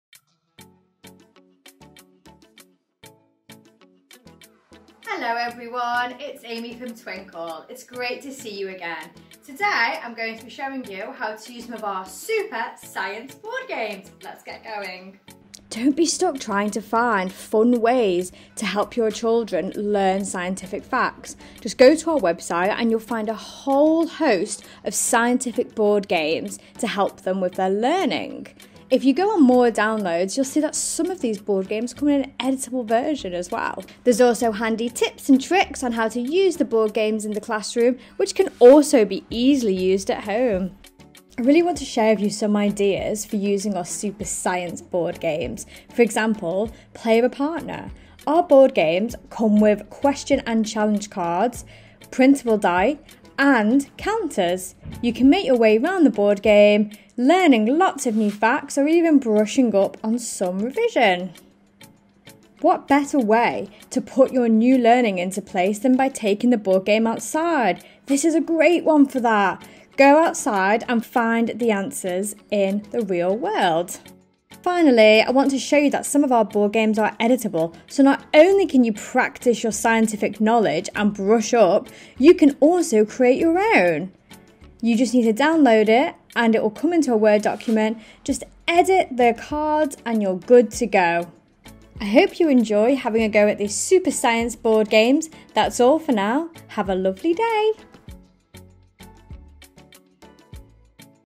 3 Super Ways to Use Twinkl's Science Board Games
Join Aimee for some fun and engaging ways to use Twinkl's Science board games.

Board games are a great way to improve memory, communication and teamwork!